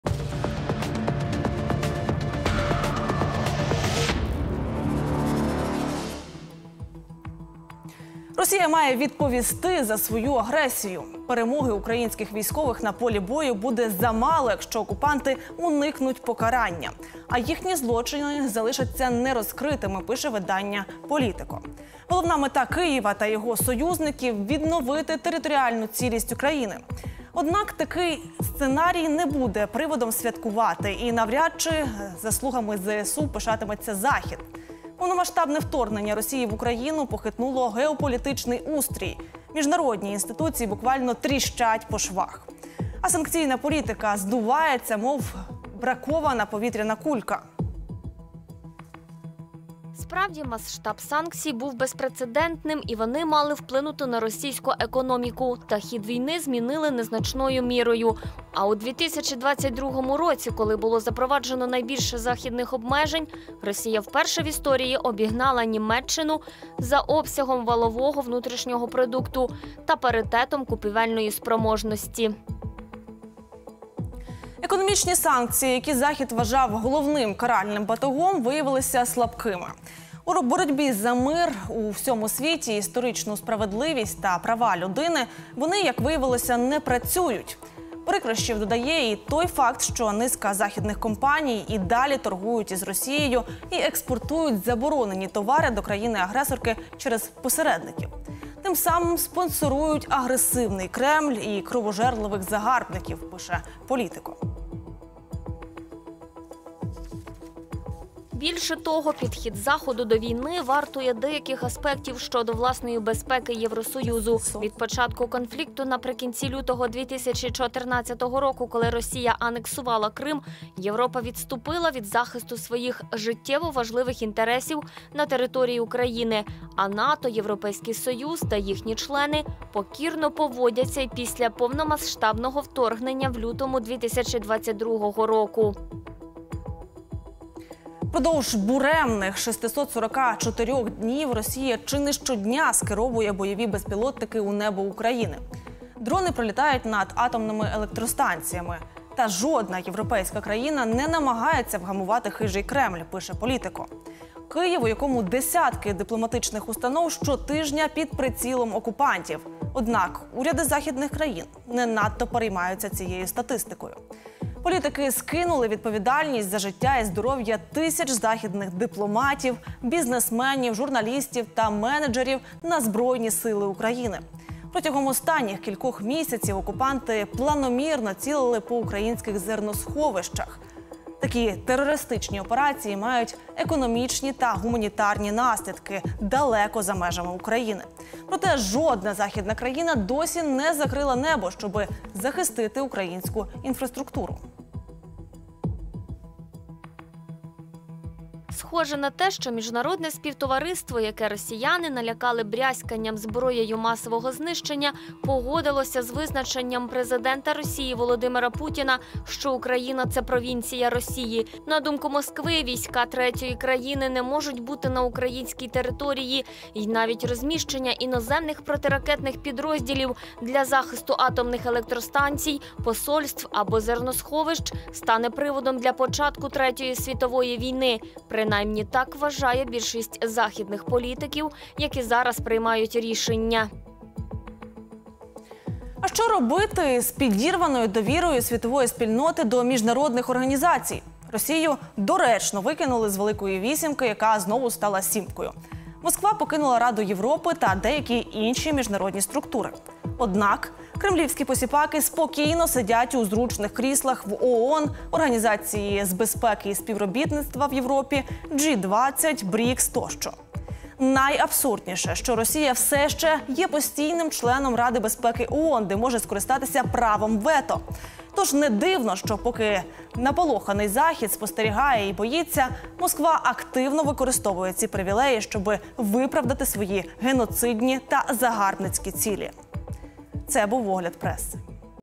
Захід боїться ймовірного початку Третьої світової війни. Огляд міжнародної преси
Команда UWN проаналізувала статтю видання Politico, в якій йдеться про те, що Росія має відповісти за вчинки своїх військових в Україні. Перемоги українських військових на полі бою буде замало, якщо окупанти уникнуть покарання, а їхні злочини залишаться нерозкритими. Детальніше — в огляді міжнародної преси.

Долучайтеся!